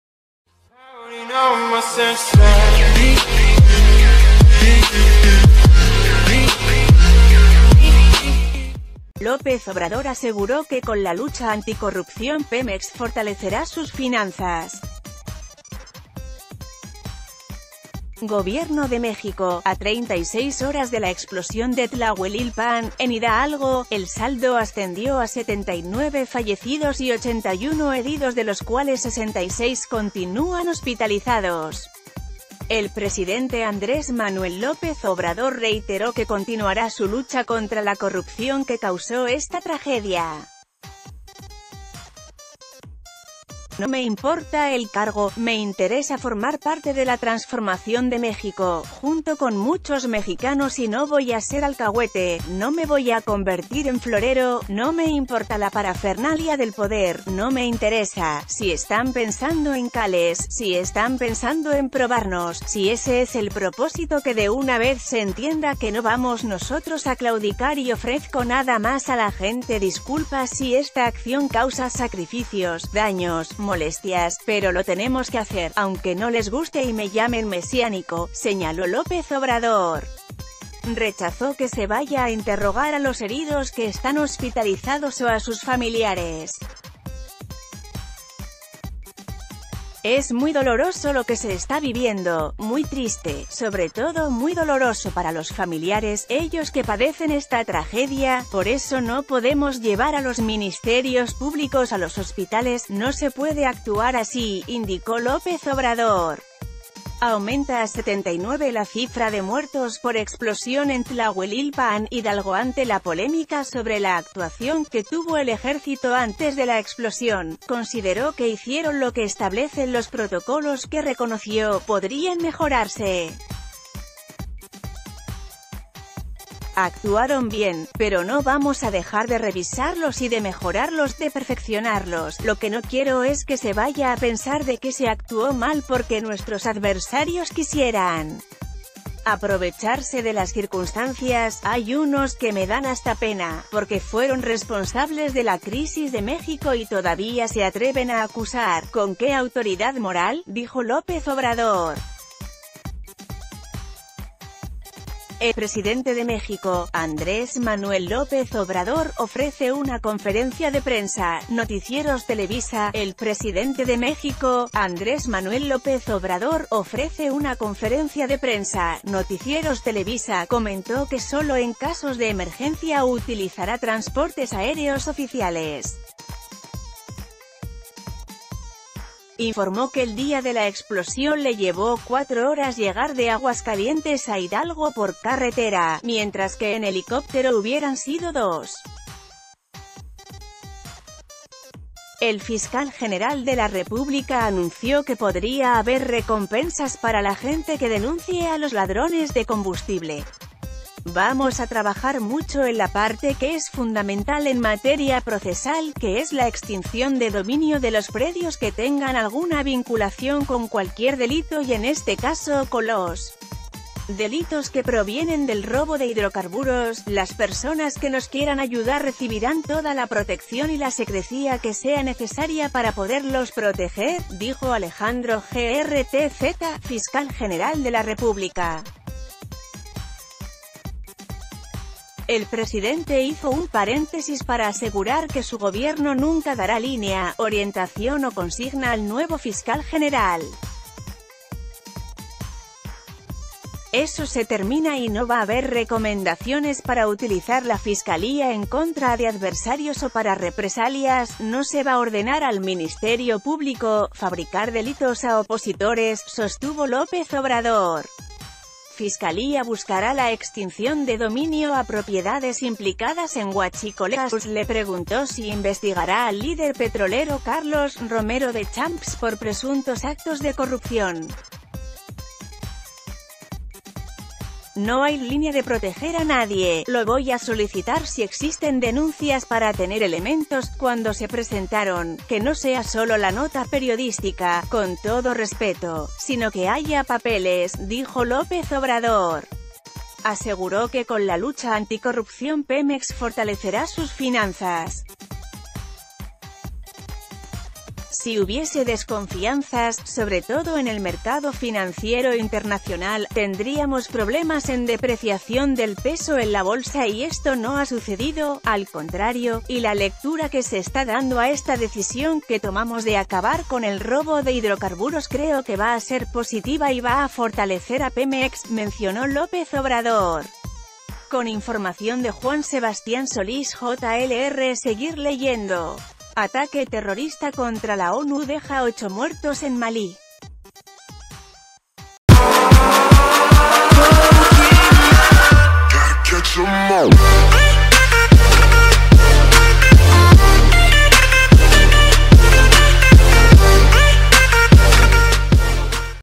Aunque nos ‘calen’ no vamos a claudicar contra la corrupción: AMLO
López Obrador aseguró que con la lucha anticorrupción Pemex fortalecerá sus finanzas. (Gobierno de México) A 36 horas de la explosión de Tlahuelilpan, en Hidalgo, el saldo ascendió a 79 fallecidos y 81 heridos de los cuales 66 continúan hospitalizados. El presidente Andrés Manuel López Obrador reite...